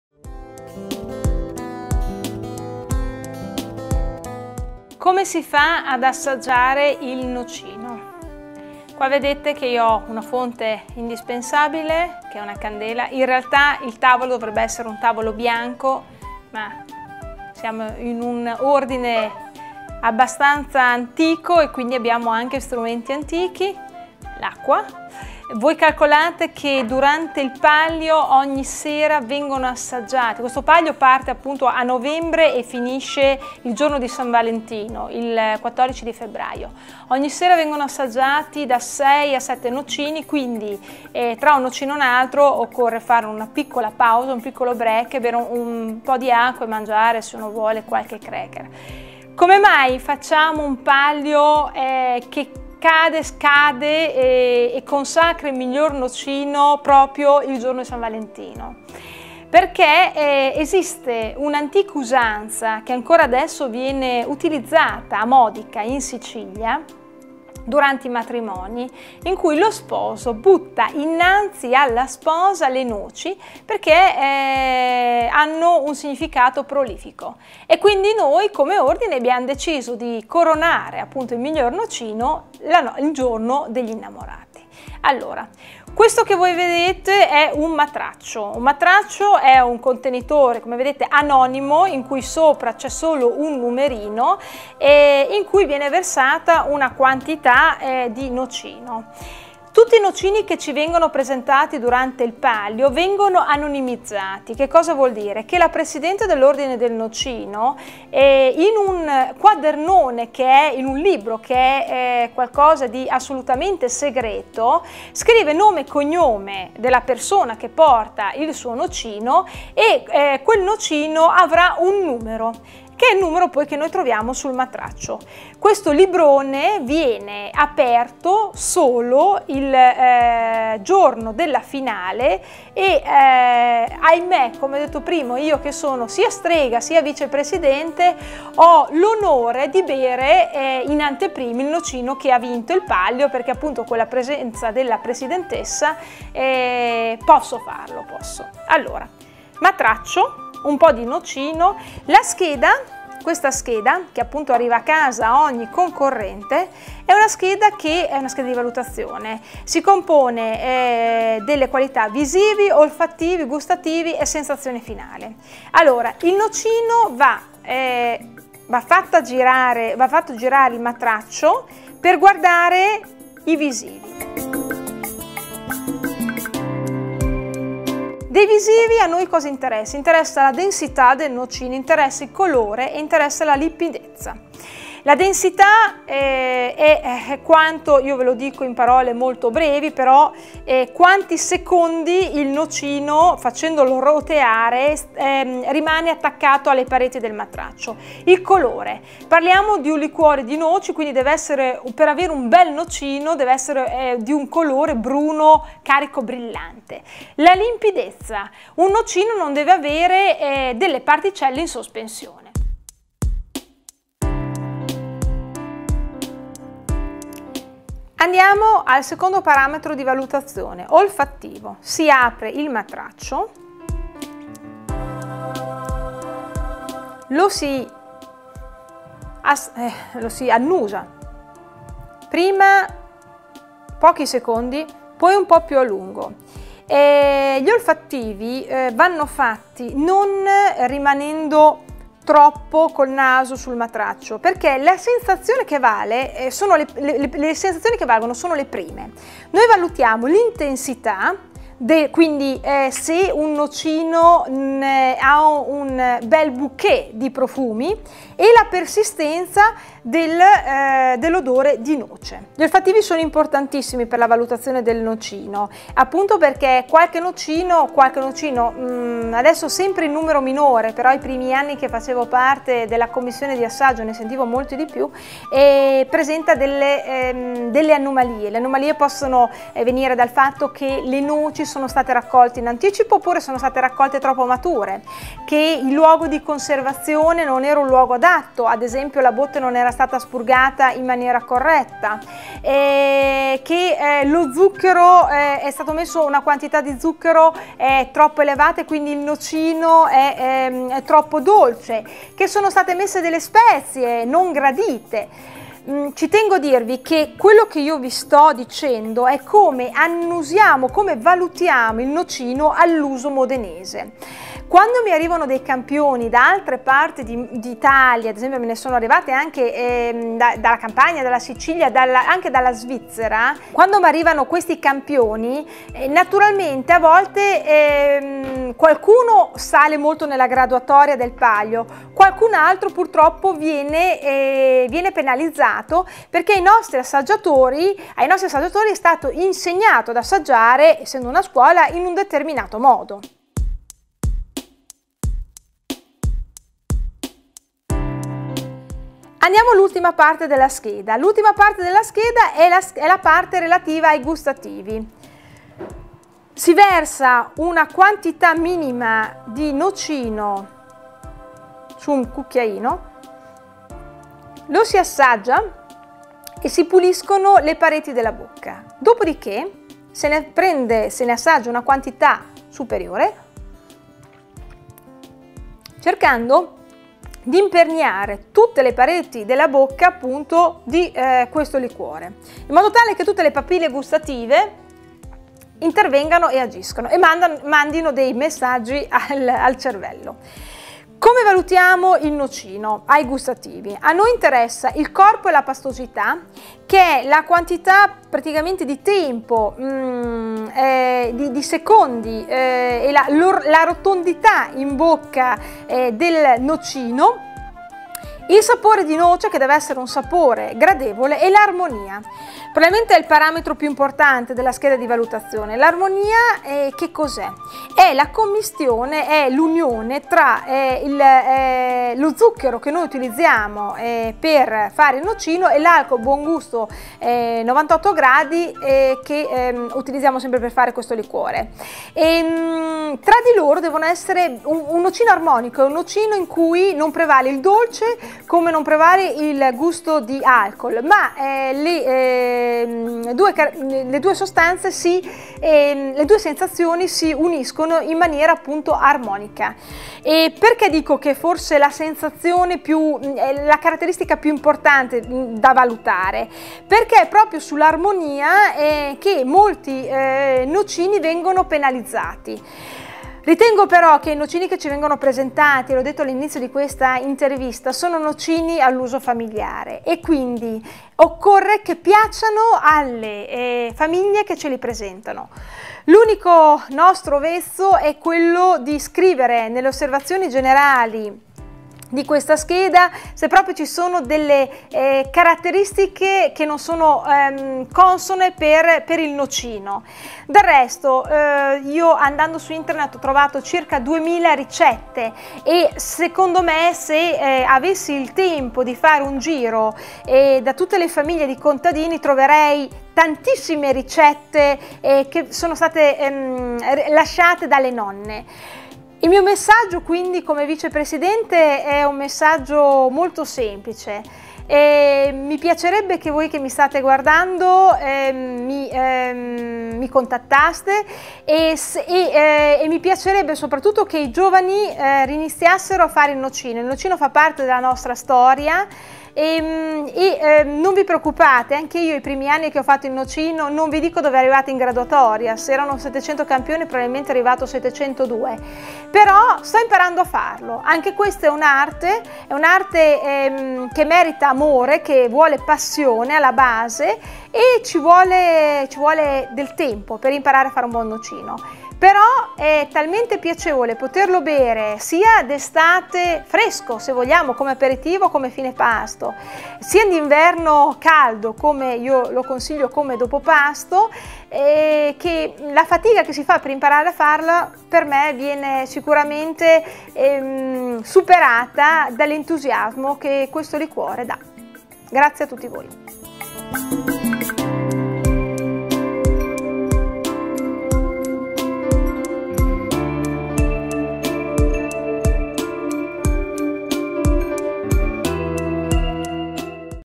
ORDINE DEL NOCINO MODENESE @ilmangiaweb SPECIALITA D'ITALIA parte due
ORDINE NOCINO MODENESE MODENA SPECIALITA D' ITALIA MODENA. L' Italia che ilmangiaweb ama raccontare Giovanni Mastroianni